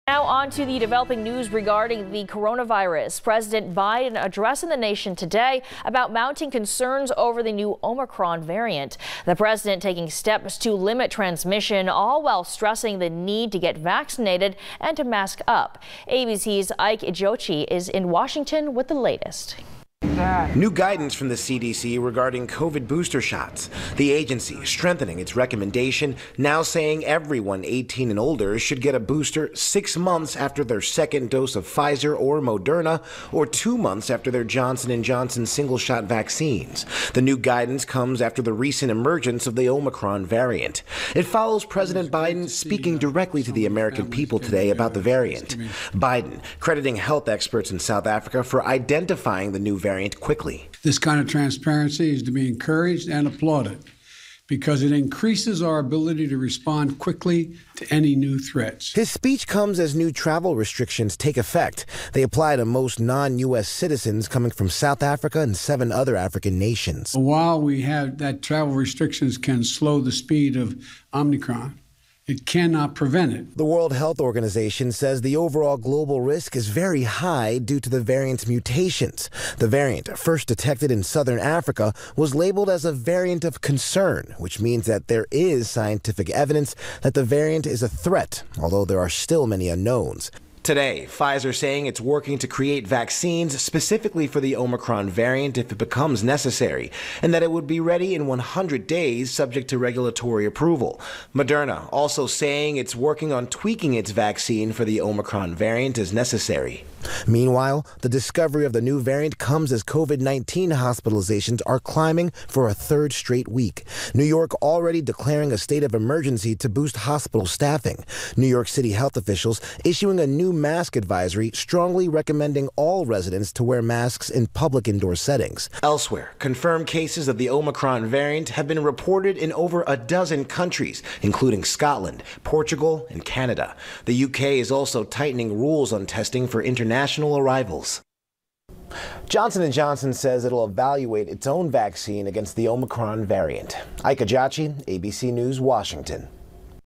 President Biden addressed the nation about mounting concerns over omicron variant
President Biden addressed the nation today about mounting concerns over the new omicron variant.  The president is taking steps to limit transmission all while stressing the need get vaccinated and to mask up.